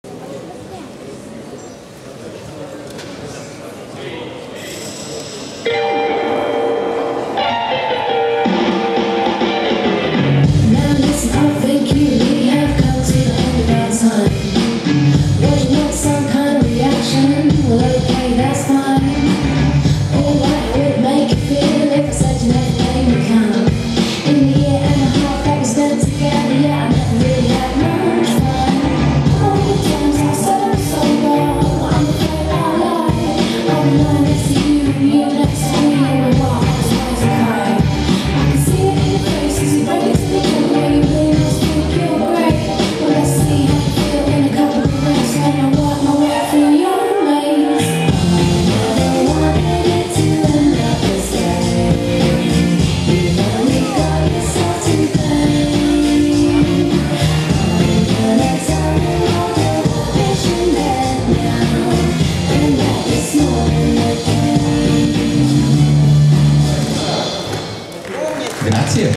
Tatiana Semenova - Competitor No 2 - Miss Figure 1 - Prejudging - NABBA World 2013
Tatiana Semenova
Competitor No 2
Miss Figure 1
Routine
Posing
Prejudging
NABBA World 2013
Bodybuilder